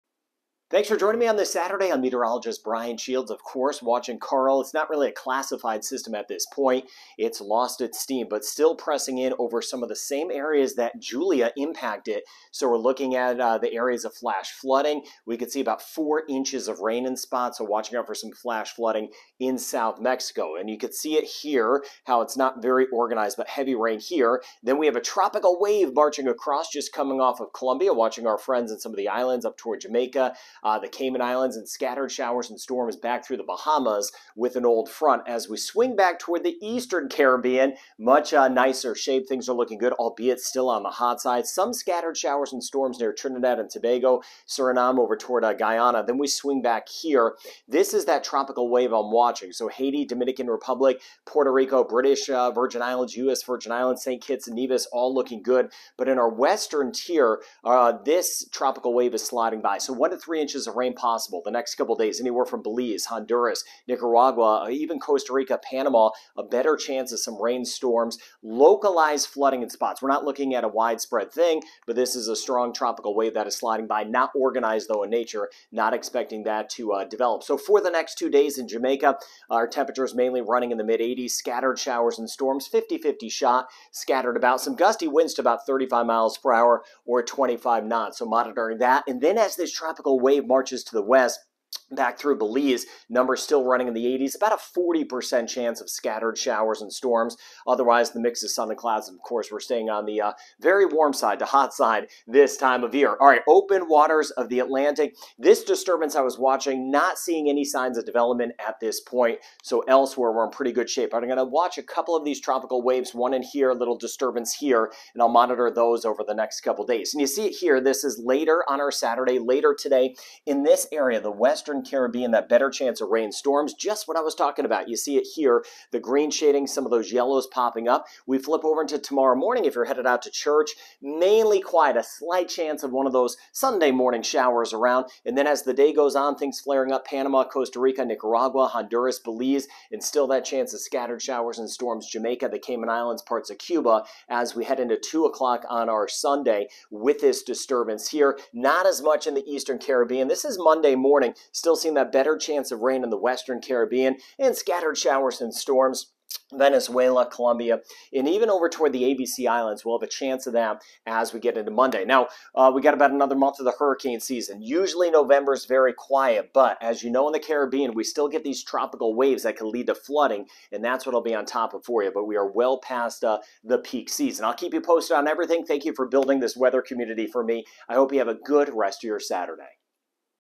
Tropical Wave Enters the Western Caribbean
A tropical wave enters the Western Caribbean, bringing a better chance of scattered rain and thunderstorms to Jamaica, The Cayman Islands, Belize, Honduras, Costa Rica, and Panama.

Tropical Storm Karl fell apart, but flooding rain continues in parts of Southern Mexico.

A couple new tropical waves will move into the Caribbean for the week ahead.

Thank you for being part of this weather community! - Brian